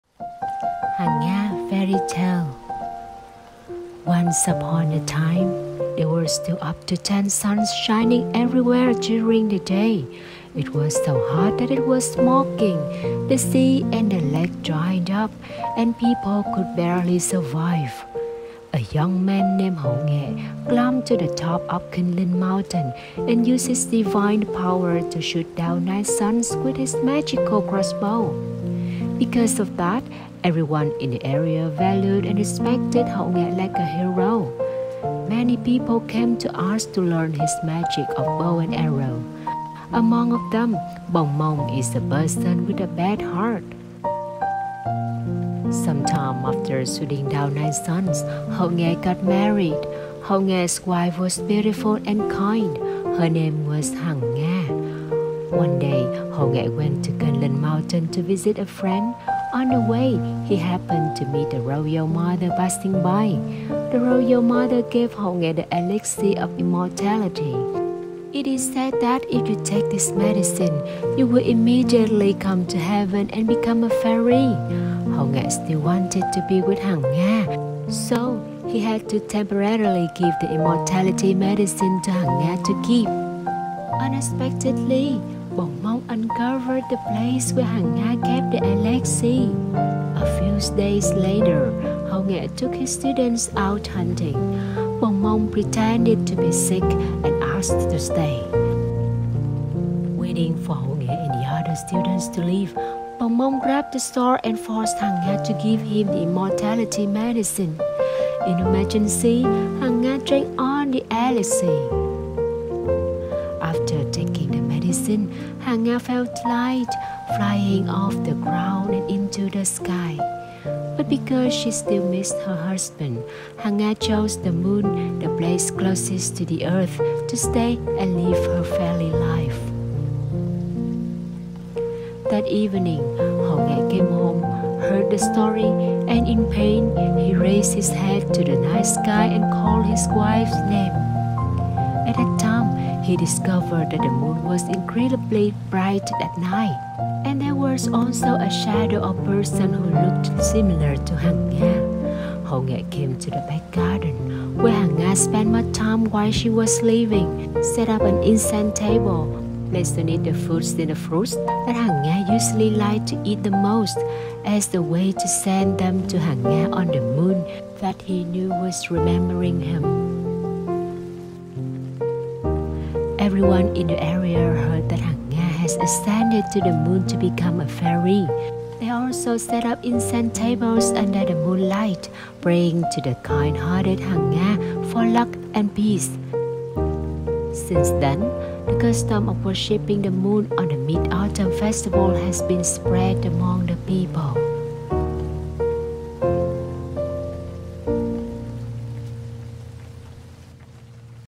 Hang Nga Fairy Tale | Januaryrange podcast K meoikechuyen book haulbooktok
Mommy, tell me a story! - A bedtime story project.
Story: Hang Nga Fairy Tale
Storyteller: K

reading vlog Januaryrange bilingual storyJanuaryrange read with me